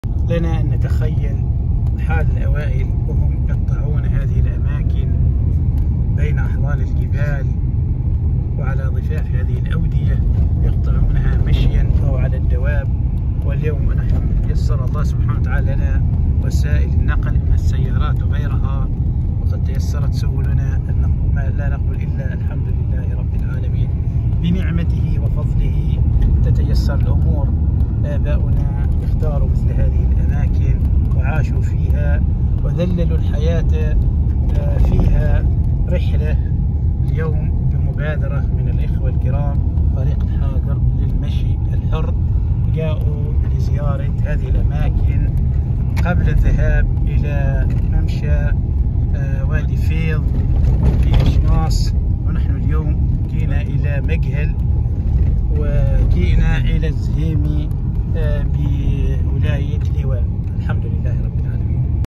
الطريق إلى بلدة_مجهل_والزهيمي.
الرحاة ضمن برنامج
المشاركة في مسير وادي_فيض.
تنظيم فريق: شناص_للمغامرة_والترحال.
السبت
٥/ شعبان/ ١٤٤٤هـ، ٢٥/ فبراير/ ٢٠٢٣